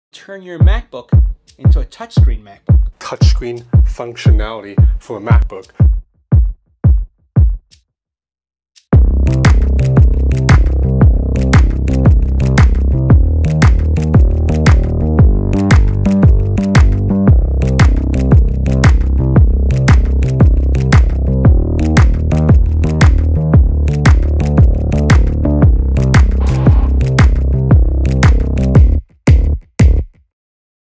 Introducing touch on Mac - espresso
Introducing touch functionality on Mac. What can you do with touch?